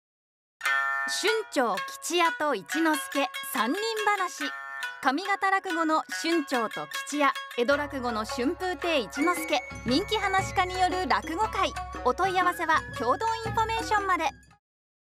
春蝶・吉弥と一之輔 3人噺 SPOT映像
春蝶・吉弥と一之輔　3人噺

出演：桂春蝶／桂吉弥／春風亭一之輔
日時：2022/04/04(月)18:30
会場：シアター・ドラマシティ